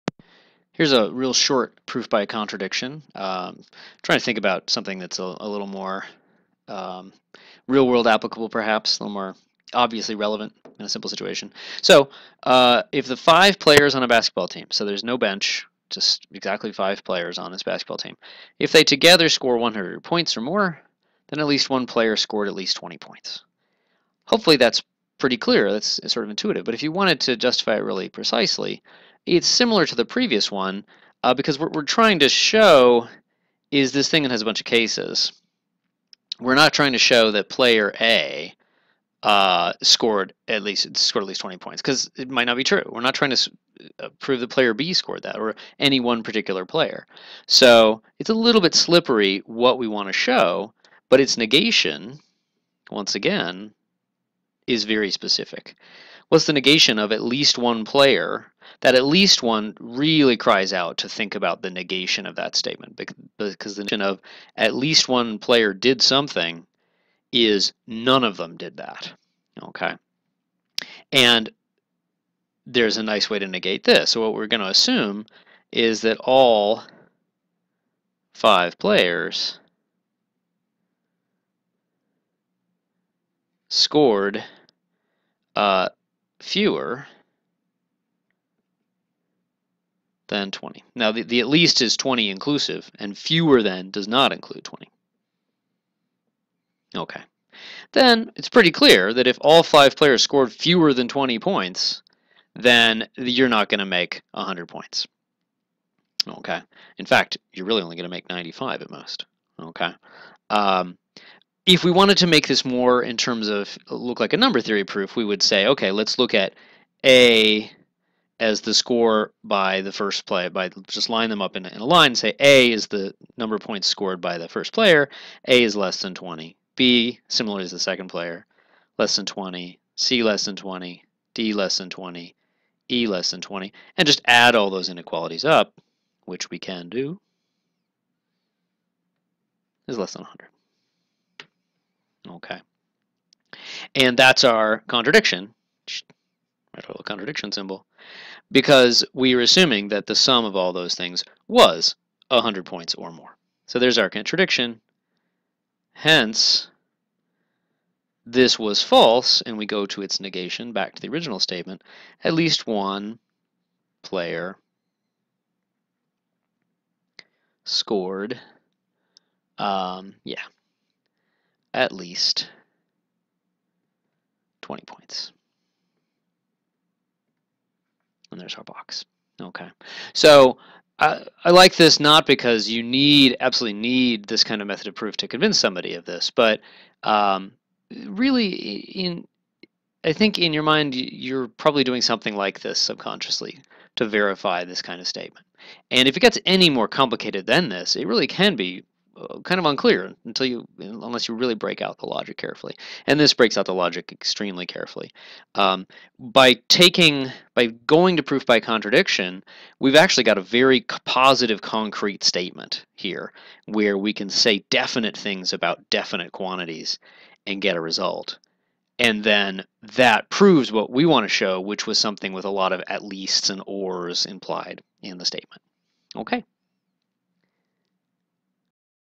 Proof by Contradiction Examples (Part 3)
A quick proof about basketball scores. This one is an example of something that may not seem to need a formal proof, but it's good to see what one looks like in such cases.